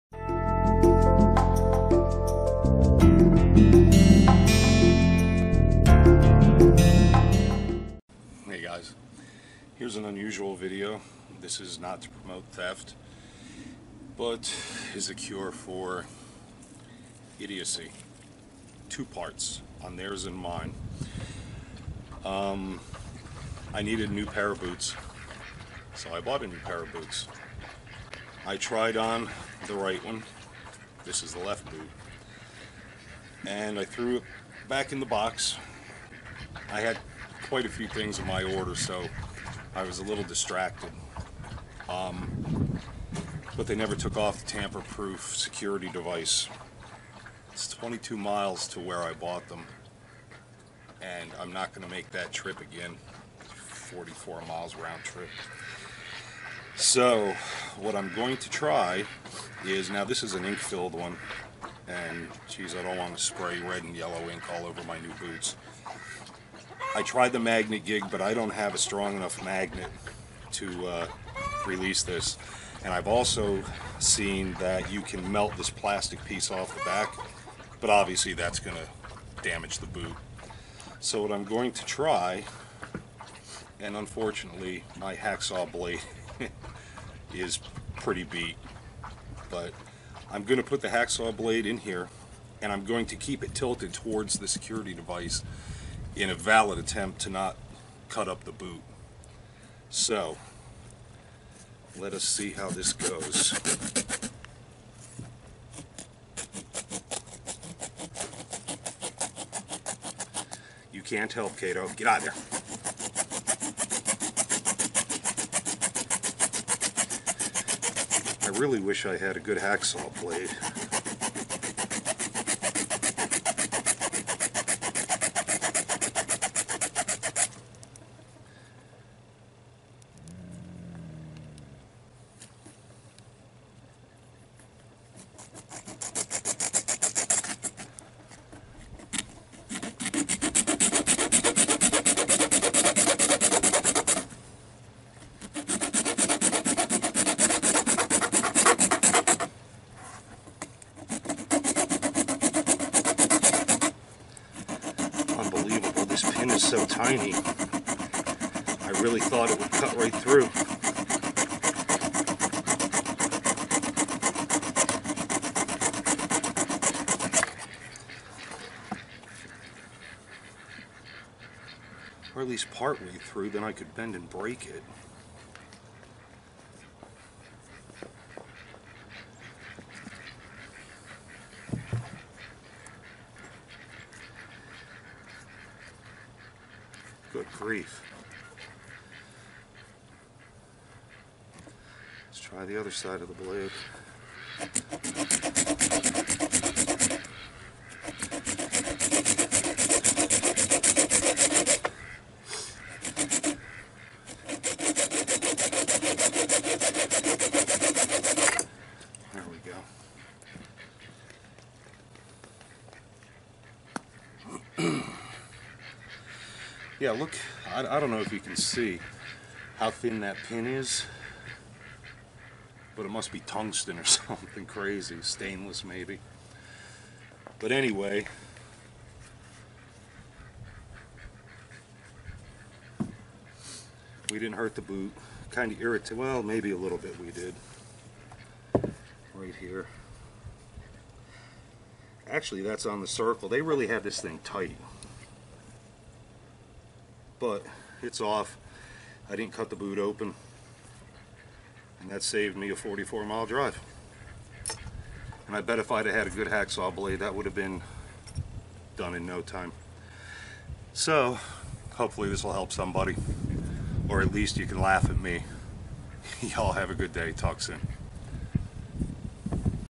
New Boots and Missed Security Tag Removal
Bought new boots and missed the security tag. Here's my solution for removing it!